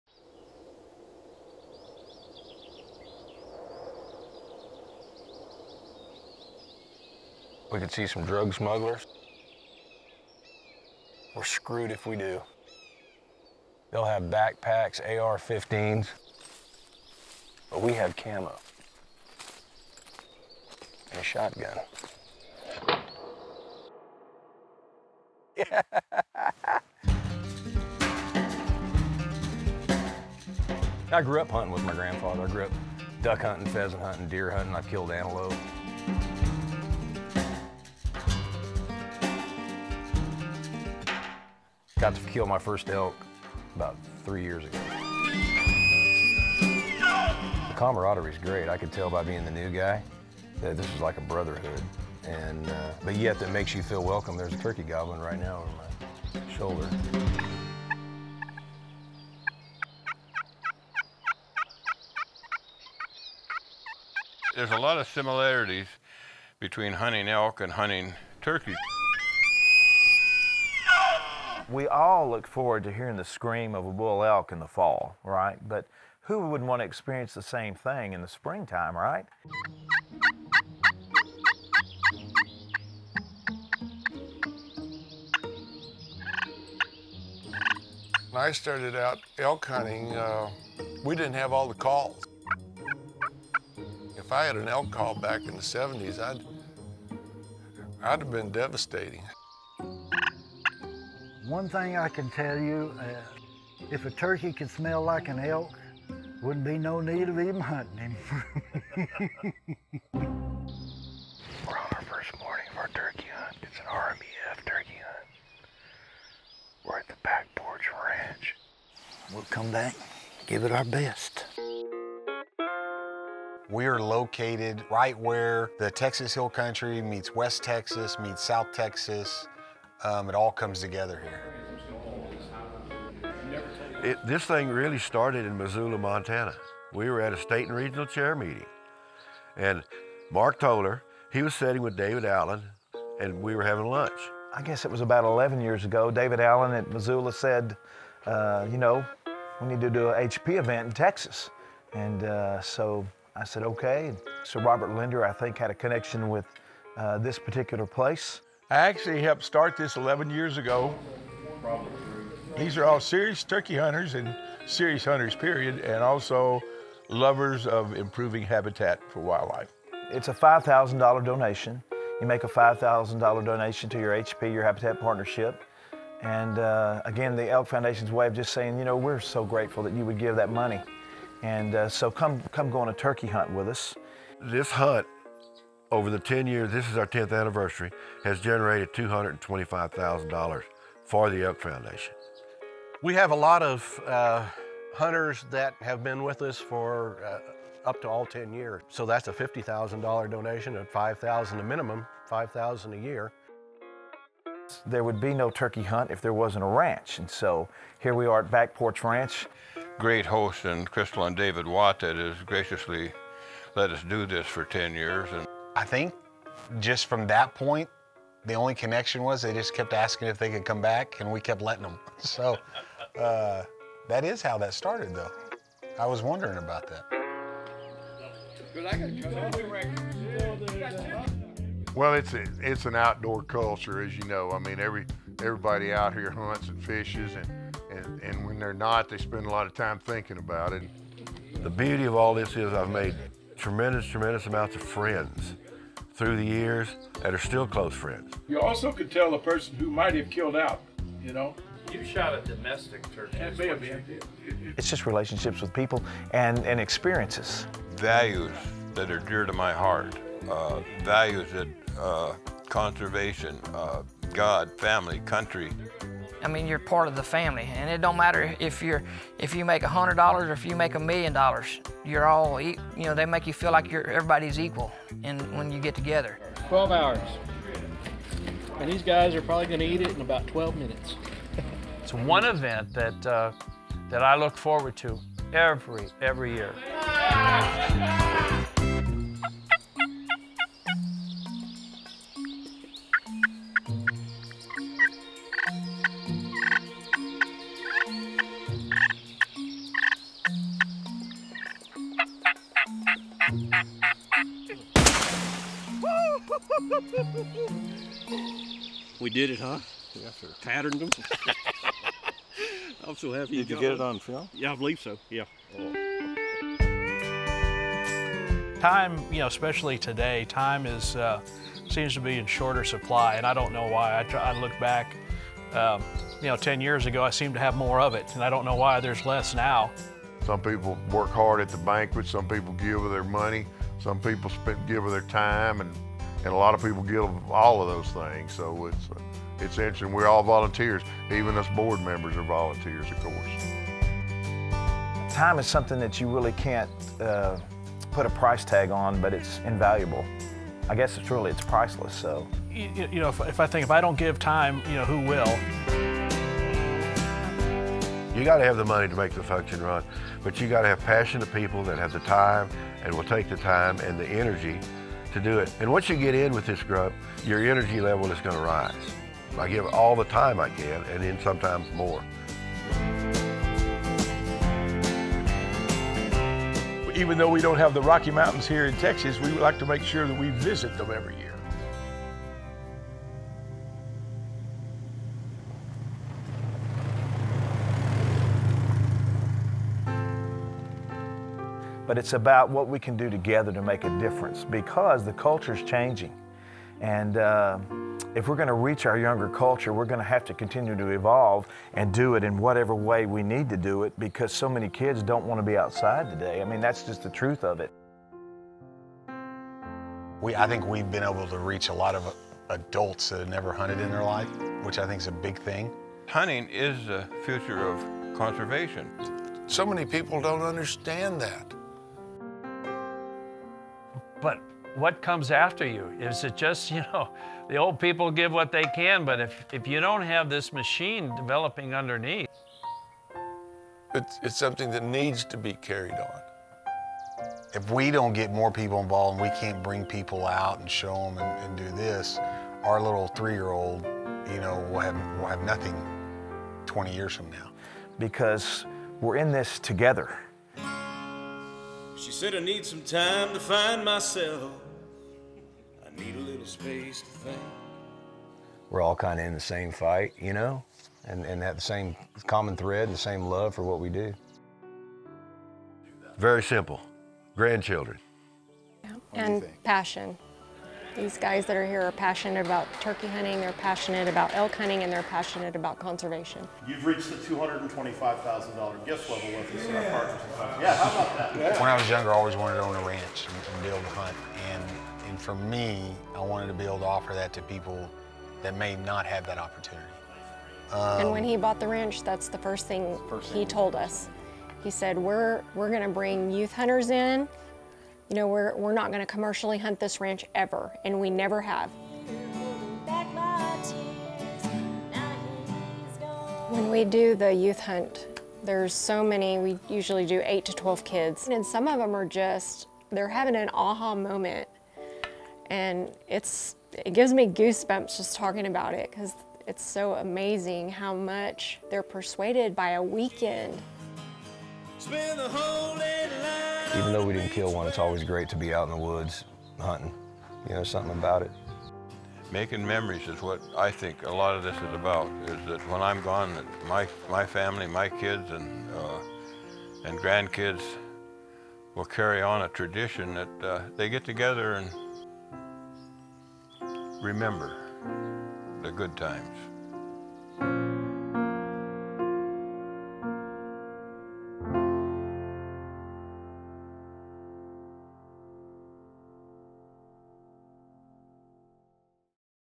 Spring Tradition - A Texas Turkey Hunt
There's no denying that turkey hunting is as close to elk hunting as you can get in the spring. Patterning, stalking and calling all come in to play for both quarries. For 10 years running, RMEF volunteers David and Crystal Watts have donated the use of their Texas ranch for youth hunters and to help RMEF donors and volunteers come together to raise funding for improving elk habitat. The event is now much more than simply a turkey hunt, it's become a tradition.